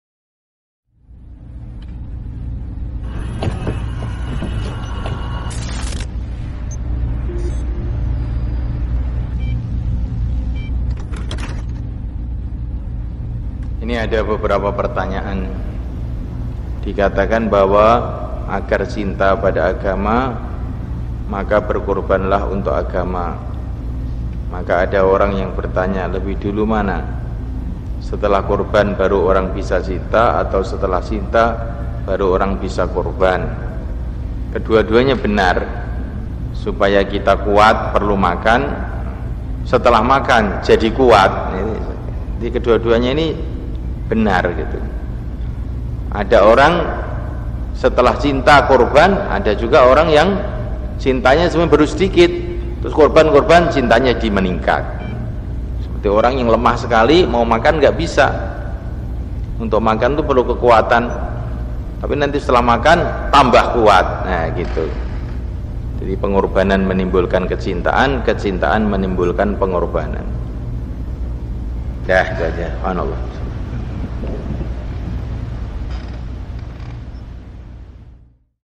Cinta Kemudian Berkorban Atau • KH. Uzairon • Al Fatah Temboro
Ma'had Darul Ulum Al Fatah Temboro, Karas, Magetan, Jawa Timur ۞ Al Mufassir Ad Dai'e Al Waalid Syekhul Uzairon Thoifur Abdillah rahmatullah'alaih